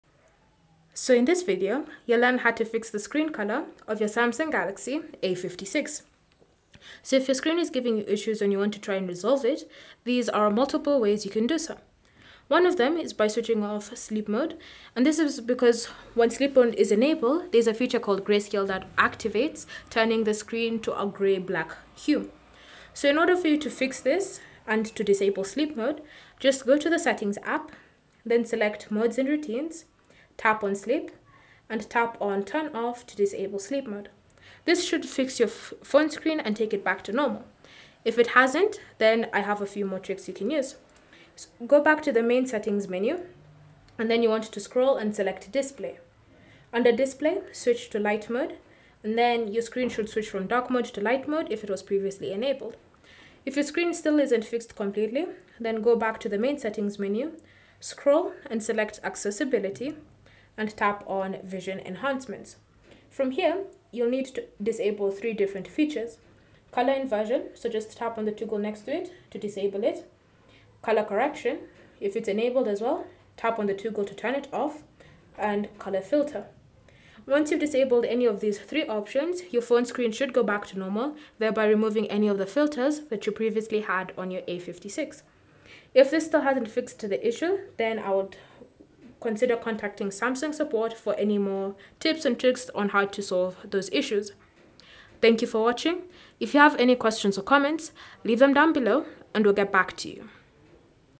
How To FIX Screen Color Of Samsung Galaxy A56
In this guide, we’ll show you how to fix the screen color on your Samsung Galaxy A56, so you can enjoy a more vibrant, accurate display. Whether you're dealing with washed-out colors or prefer a different tone, we've got easy steps to improve your screen settings.

How To FIX Screen Color Of Samsung Galaxy A56 using different methods.
Tap on modes & routines
Select sleep
Click on turn off to disable sleep mode

If the screen is not fixed, go back to the main settings menu
Select display
Switch to light mode

If the problem persists, Go to the main menu
Select accessibility
Tap on vision enhancement
Find and disable Color inversion, color correction & color filter

Cell Phone Tripod Adapter